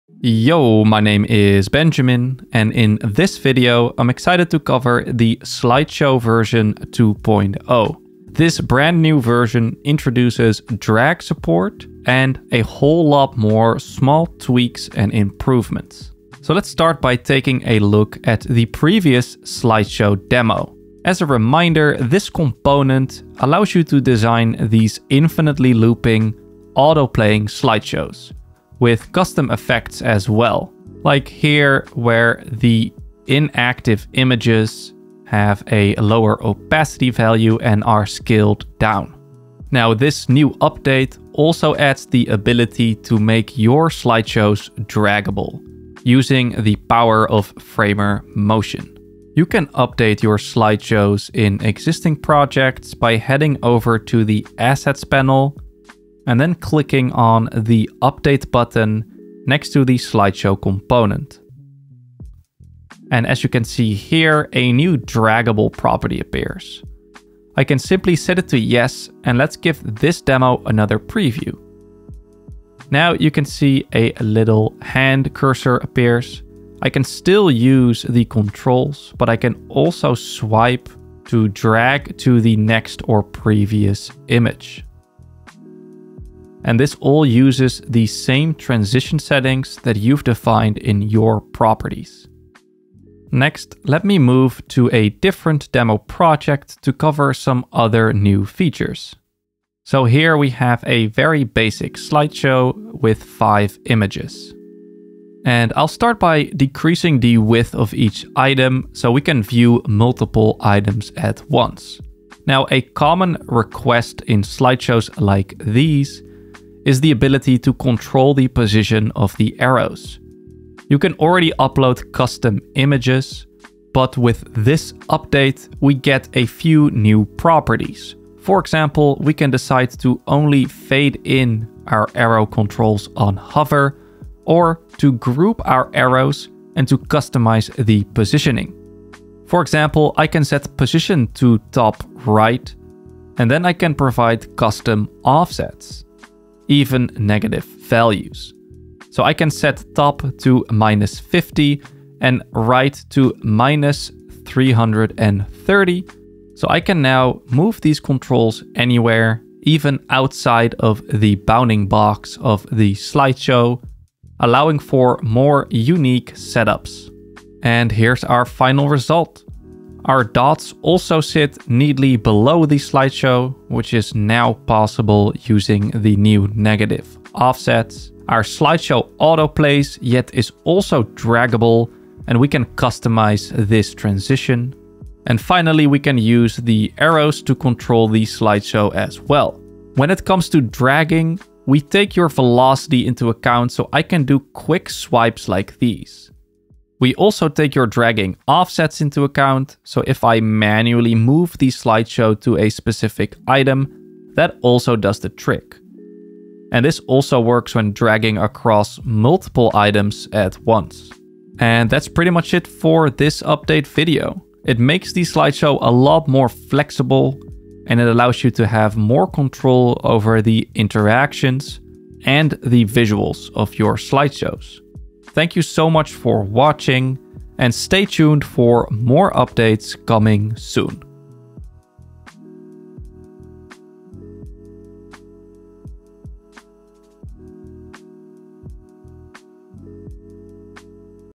Framer Update: Slideshow 2.0
This Slideshow update introduces dragging support and new layout options for your controls, plus a bunch of smaller tweaks. Create auto playing and infinitely looping slideshows that can be slowly dragged or quickly swiped. Powered by Framer Motion—so you can design your own spring transitions with ease. Find the Slideshow component in the Insert Panel, under the Interactive section. To grab this update in your existing project, go to the Assets Panel, and hit Update next to the component. Remix demo